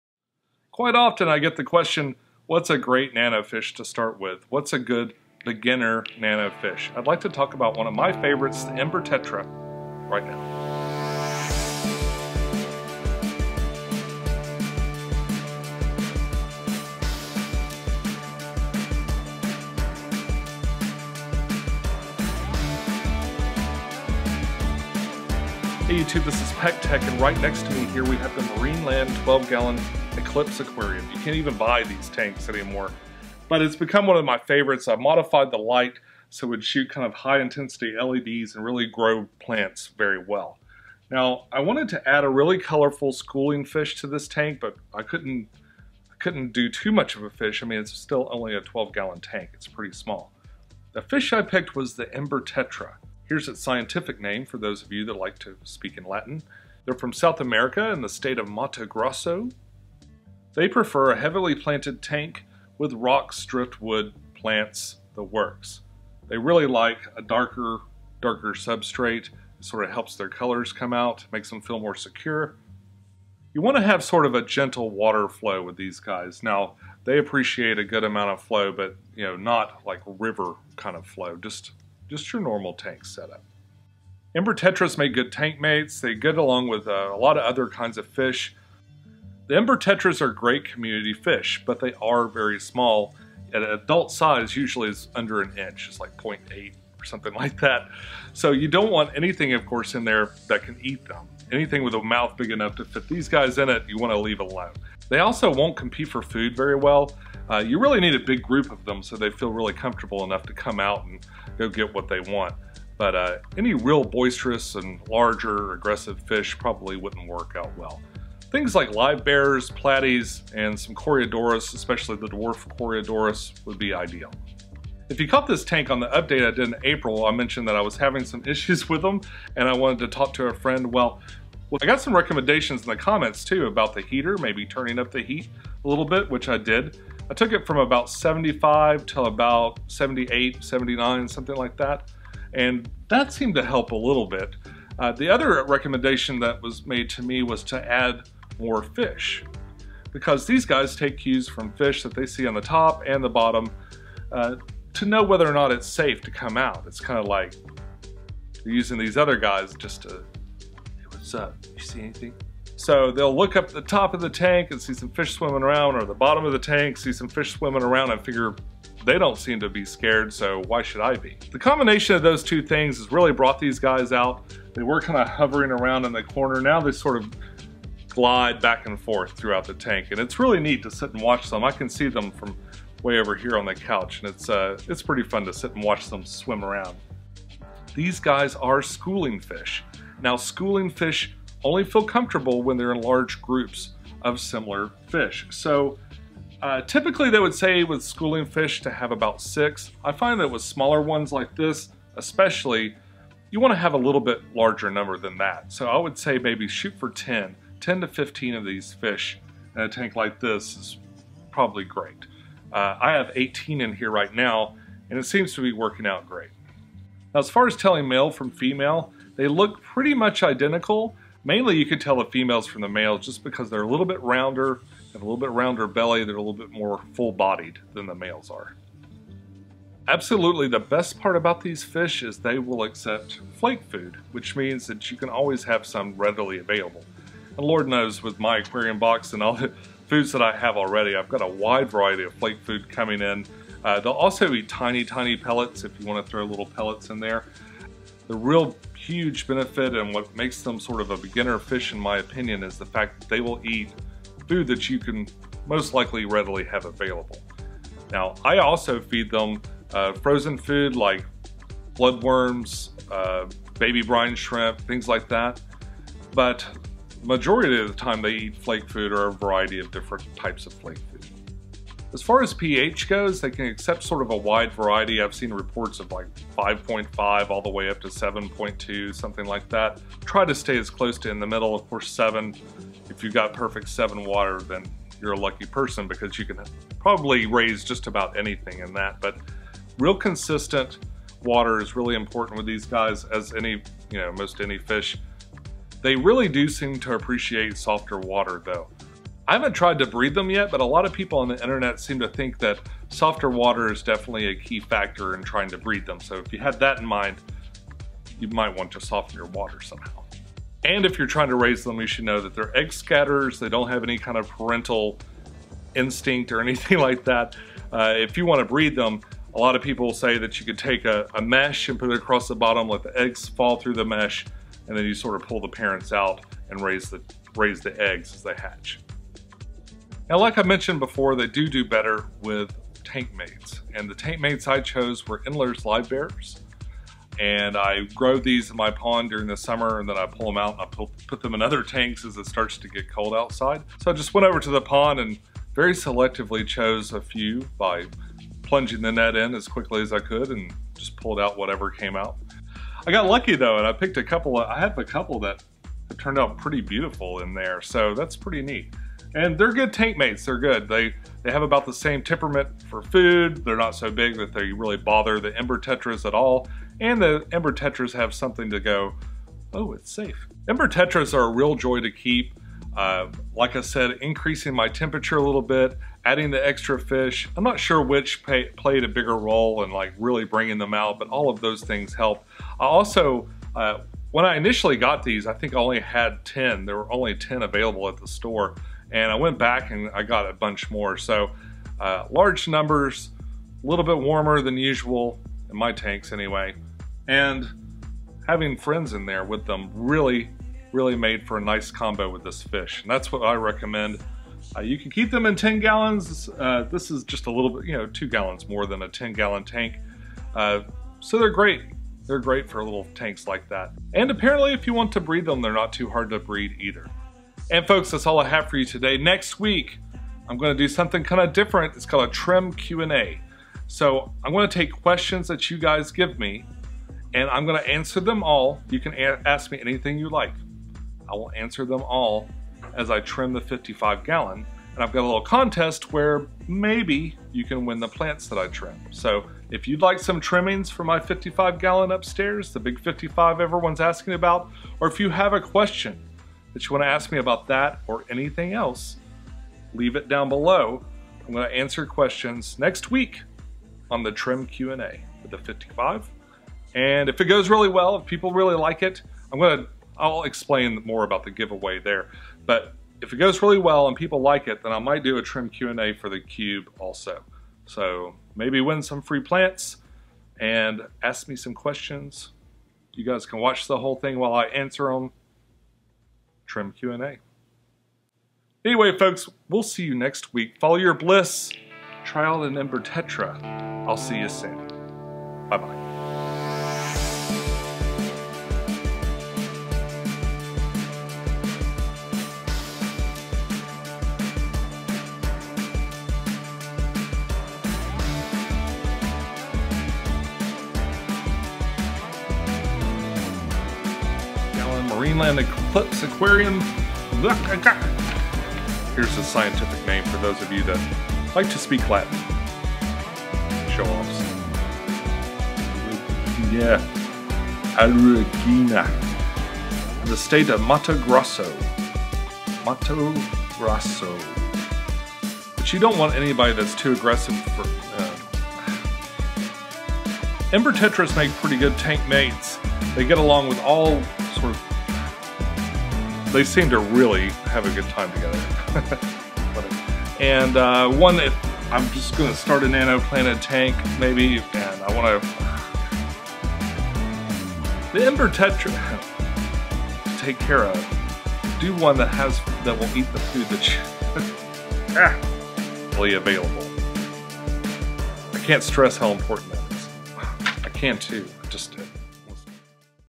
The Ember Tetra - Hyphessobrycon amandae
A favorite nano fish of mine is the Ember Tetra (Hyphessobrycon amandae). I have had a lot of fun getting to know it better and thought I would share it with you.